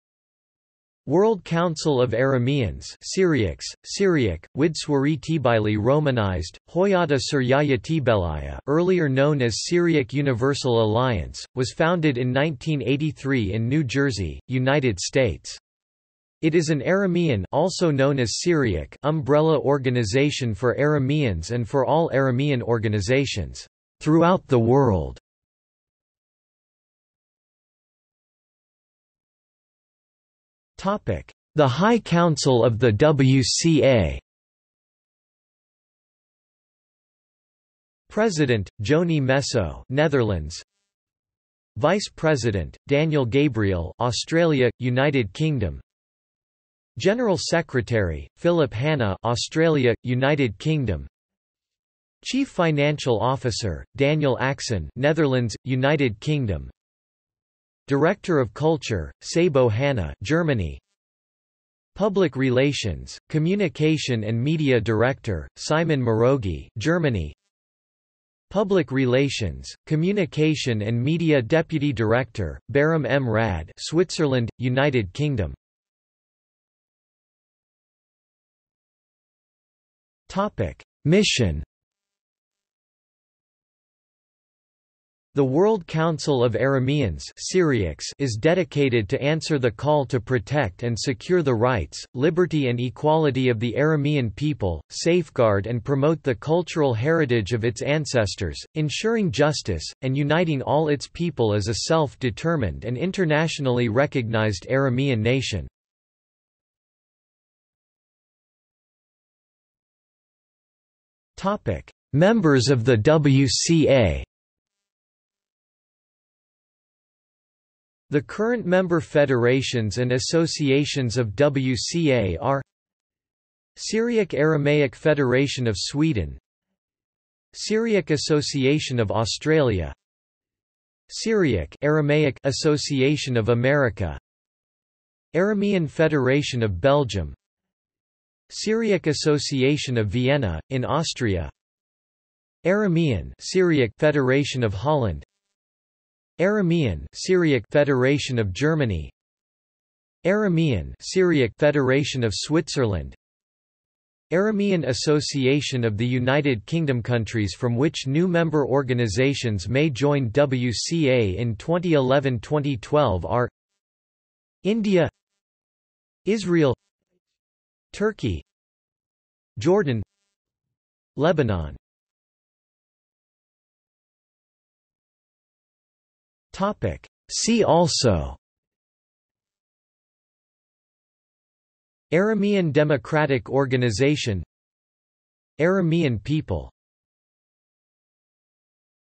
World Council of Arameans (Syriacs) | Wikipedia audio article
00:00:27 1 The High Council of the WCA
00:01:16 2 Mission
00:01:47 3 Members of the WCA
00:02:52 4 See also

- Socrates

SUMMARY
World Council of Arameans (Syriacs) , (Syriac: ܚܘܝܕܐ ܣܘܪܝܝܐ ܬܒܝܠܝܐ‎, romanized: Ḥuyādā Suryāyā Tbelāyā) earlier known as Syriac Universal Alliance, was founded in 1983 in New Jersey, United States. It is an Aramean (also known as Syriac) umbrella organization for Arameans and for all Aramean organizations "throughout the world".